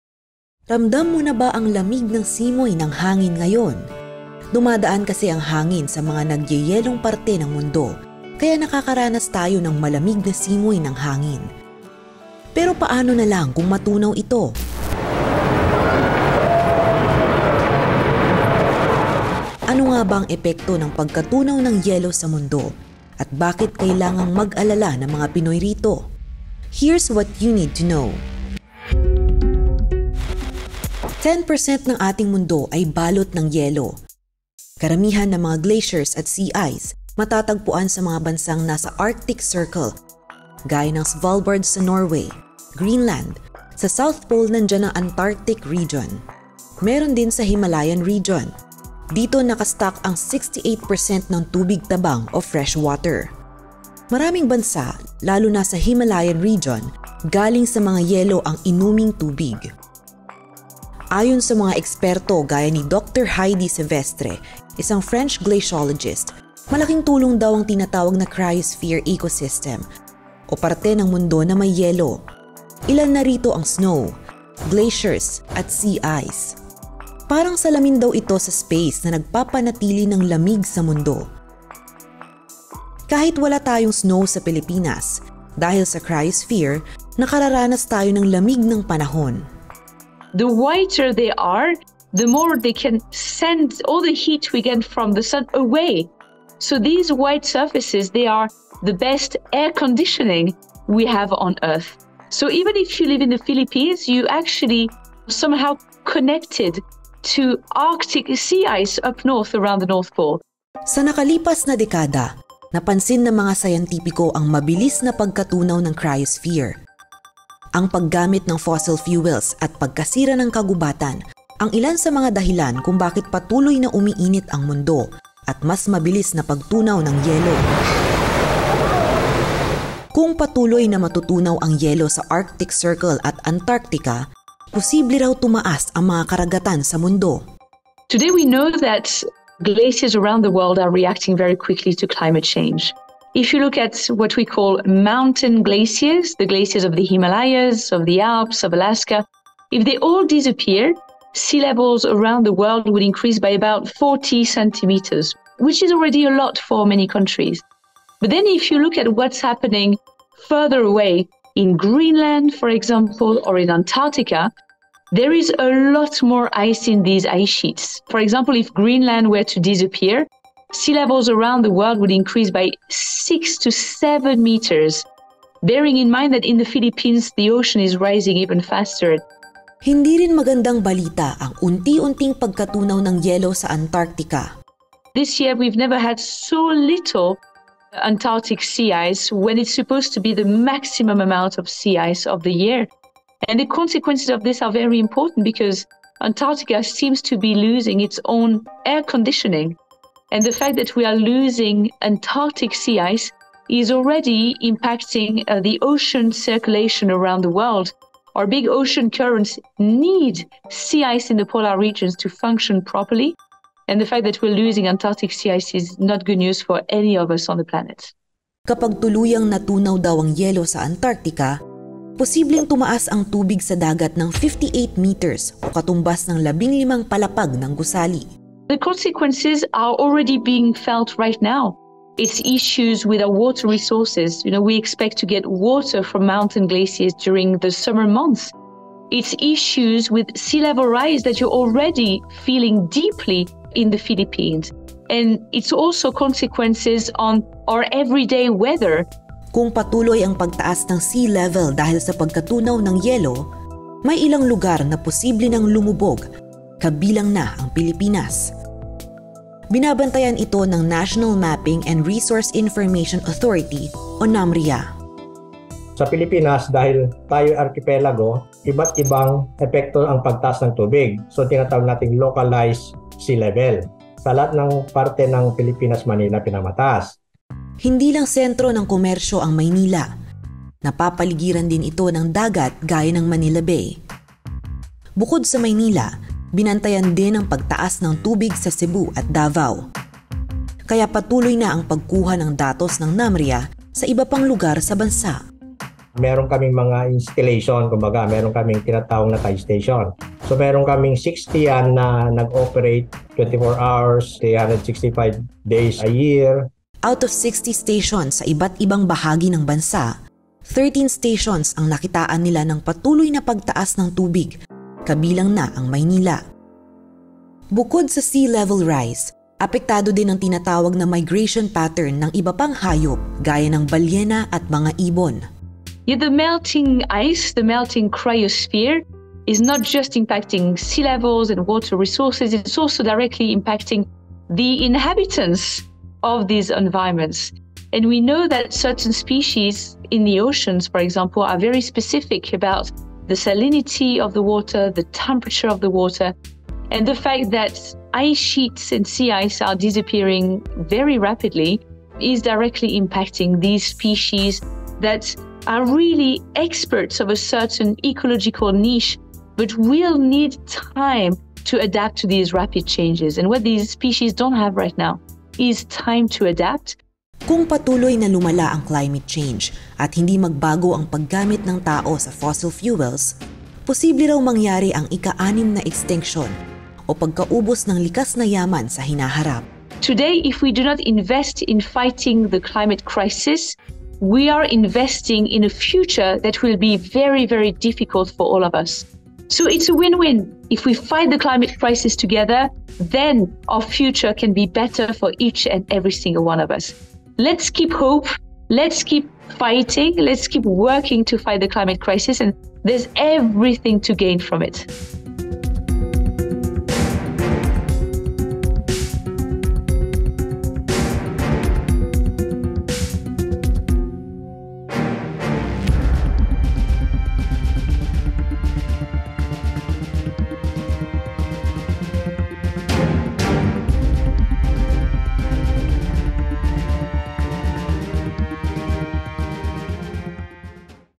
Bakit kailangan mag-alala ng mga Pinoy sa pagkatunaw ng yelo sa mundo? | Need To Know
NILALAMIG KA BA O NANLALAMIG NA SIYA SA ‘YO?

Alam mo bang 10% ng ating mundo ay yelo?

Sa nakalipas na dekada, napansin ng mga experto ang mabilis na pagkatunaw ng mga yelo sa mundo. Bakit tayo dapat maalarma sa mabilis na pagkatunaw ng mga yelo sa Antartica? Here’s what you